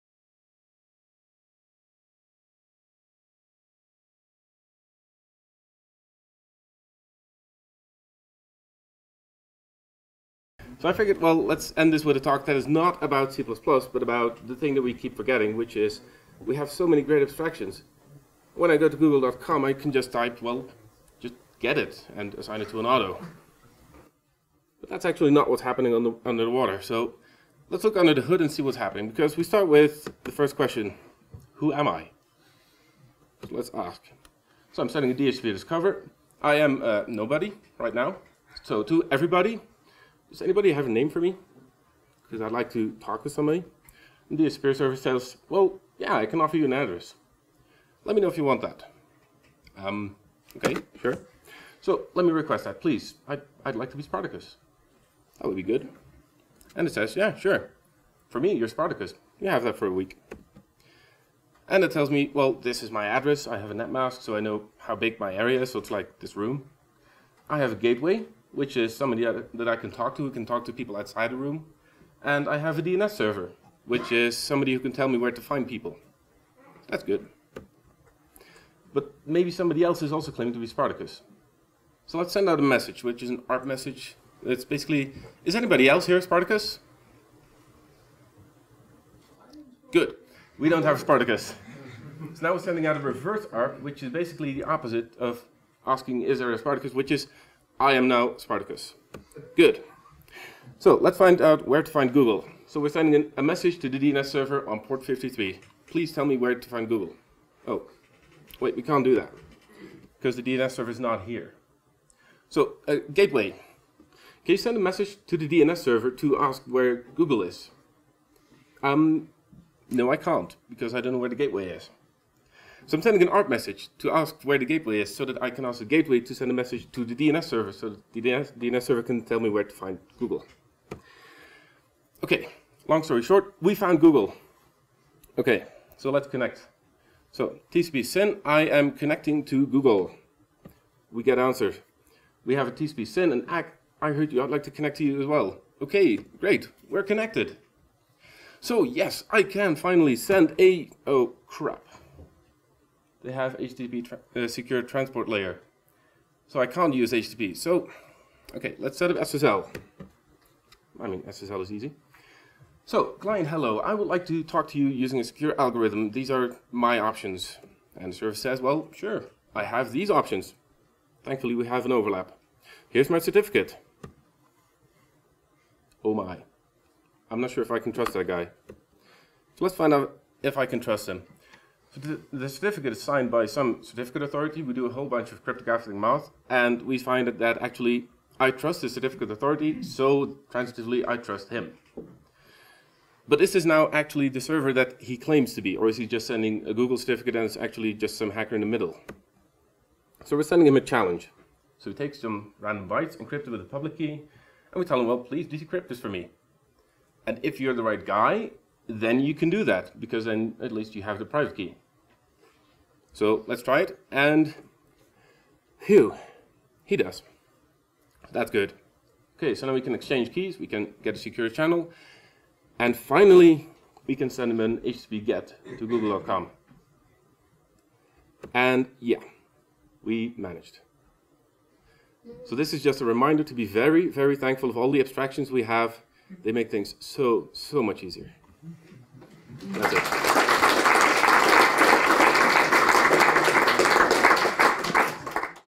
C++Now 2017: Peter Bindels “How to go to Google.com"
Lightning Talk
Presentation Slides, PDFs, Source Code and other presenter materials are available